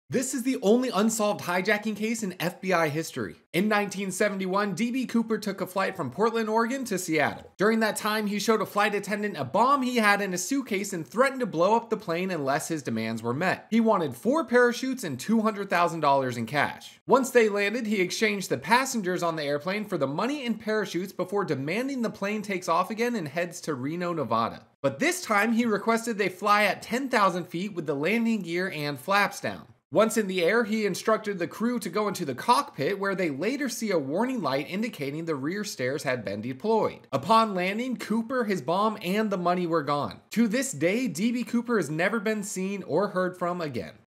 The Strange Disappearance of DB Cooper
The only unsolved hijacking case the FBI was unable to solve was that of DB Cooper, who managed to escape in an incredible way.